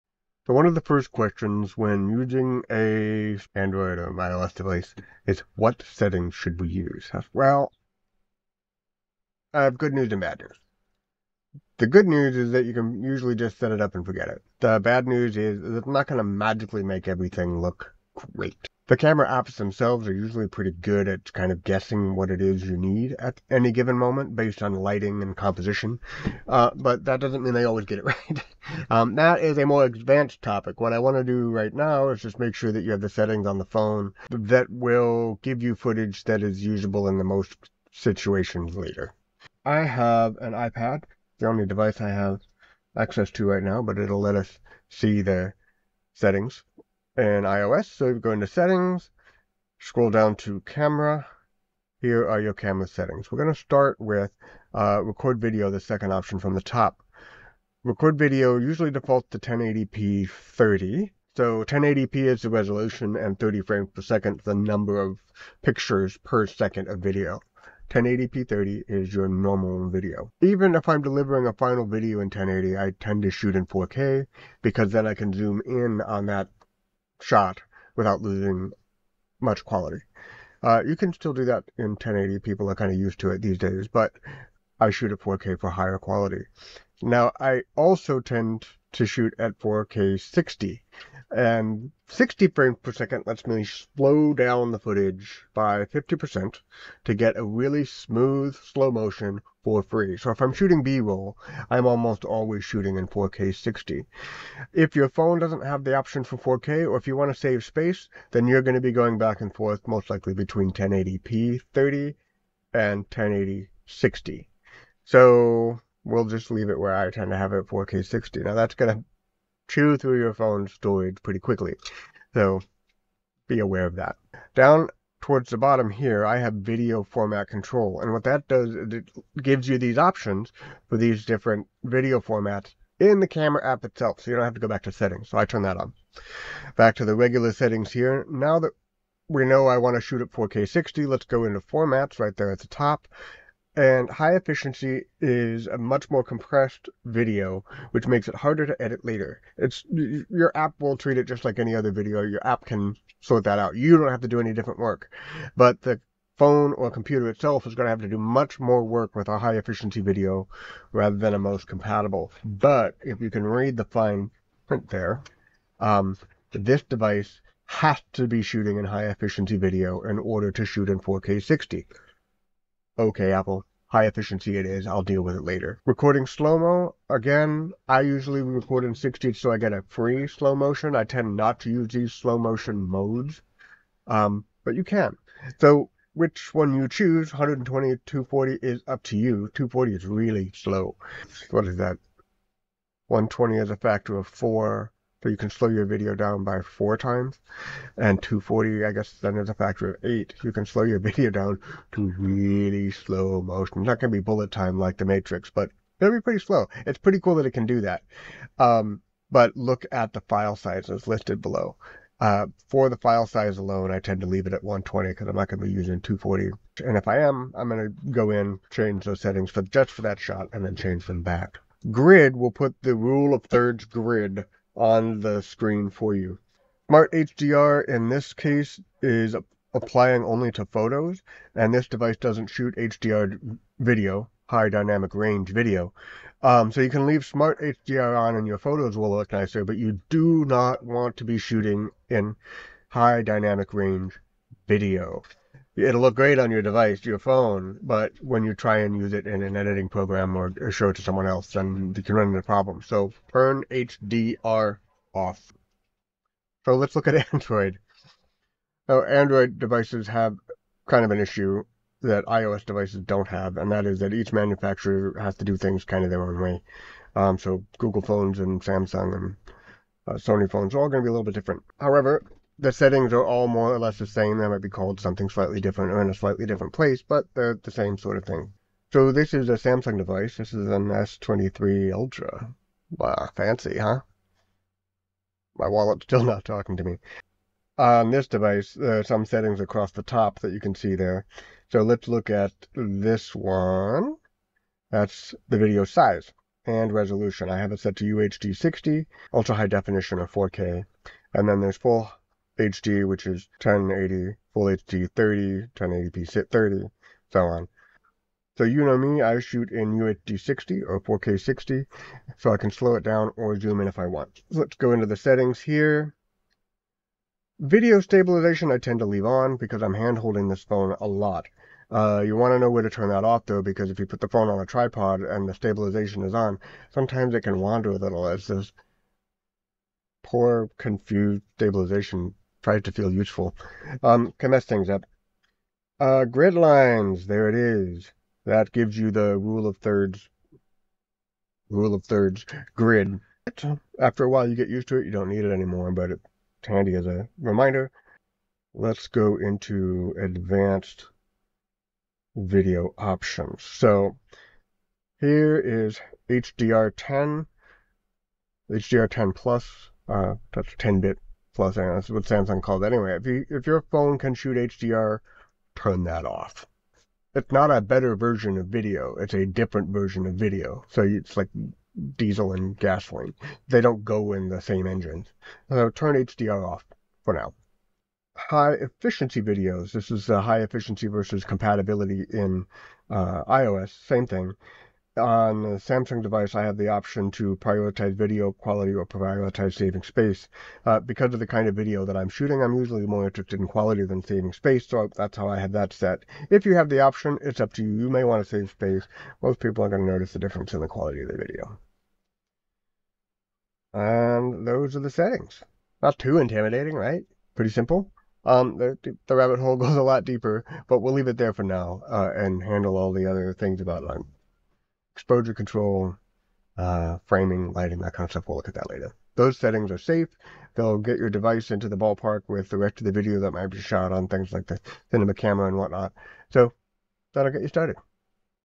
Zero Budget Media: Phone Settings
What phone camera settings are the best? The honest answer to that is "It depends on what you're shooting..." but here is a look at some general settings that will help make your video footage consistent over time.

Part of a curriculum for low budget production developed with the support of Lomikai Media and Rising Voices Changing Coasts Hub.

Production Notes:

This one was fun. The face cam is the 6K pro into the ATEM switcher. The iPhone/Android feed is going into an Acasis USB C dock with an HDMI output running to the ATEM. Picture in Picture is done via ATEM software control, ATEM is recorded in OBS. Audio is the Blue Bluebird through the Zoom H6 into OBS. Edited in Resolve.